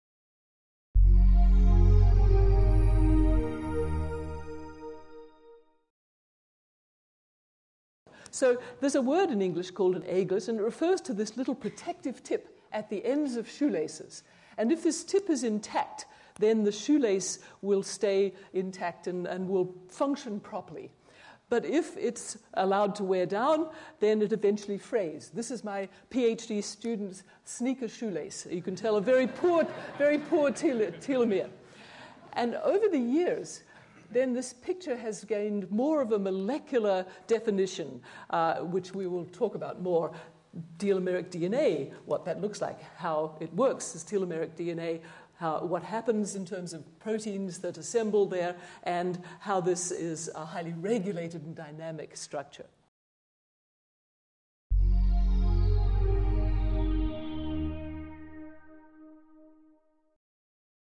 2009 Nobel Lecture in Physiology or Medicine by Elizabeth H. Blackburn
In this excerpt from her Nobel Lecture, Elizabeth H. Blackburn compares her student´s shoelace with a poor telomere.